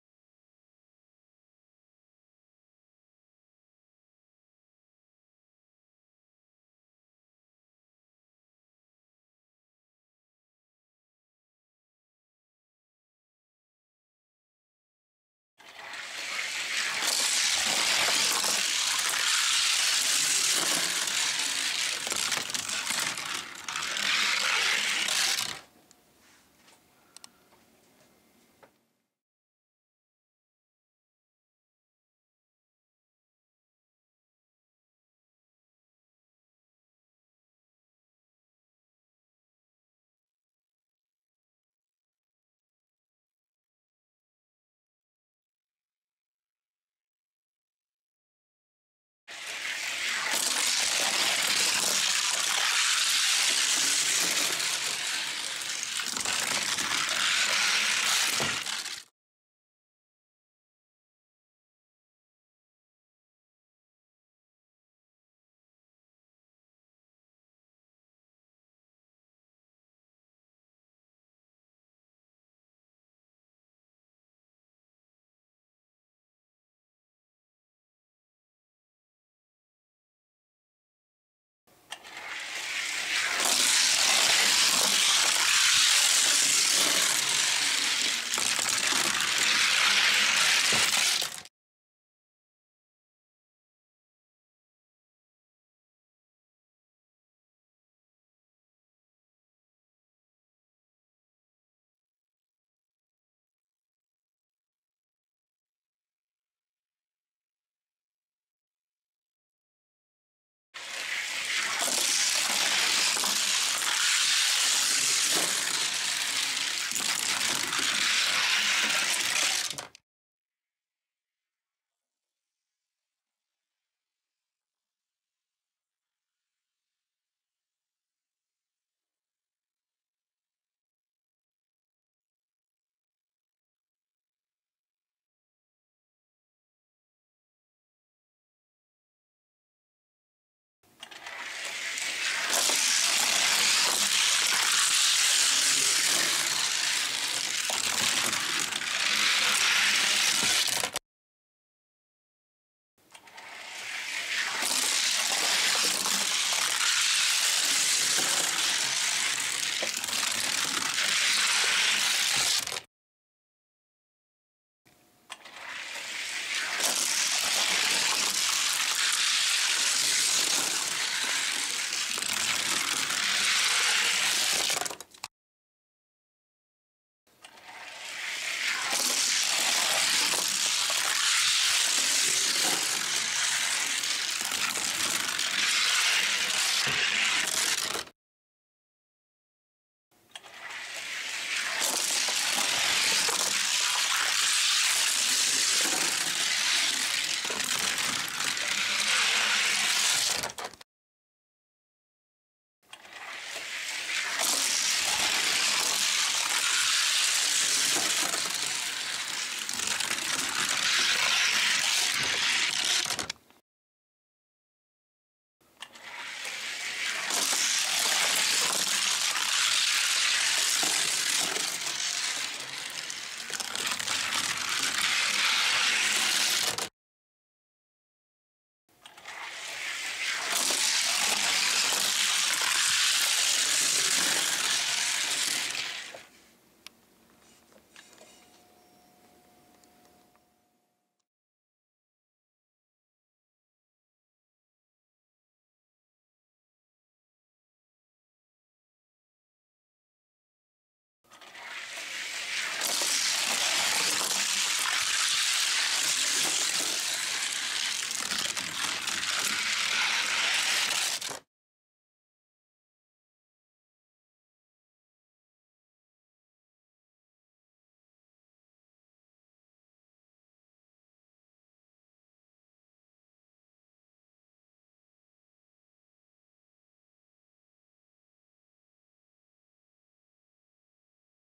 Die-Cast Racing - 2016 Formula R Challenge - Champion's Stage - Race 4
Event:  FR Challenge (Champions)
Round:  1   Race:  4
Cars:  HW500 (35g) vs. Silver 2011 Indy Car (44g)
Teams:  Gravelocity vs. Hi-Playin Drifters

The last race of the first round is between the HW500 from team Gravelocity and the silver and white 2011 Indy Car from the Hi-Playin Drifters.  Check out the video to see which one will join the other three in the semi-finals. Come back tomorrow for the start of Round 2!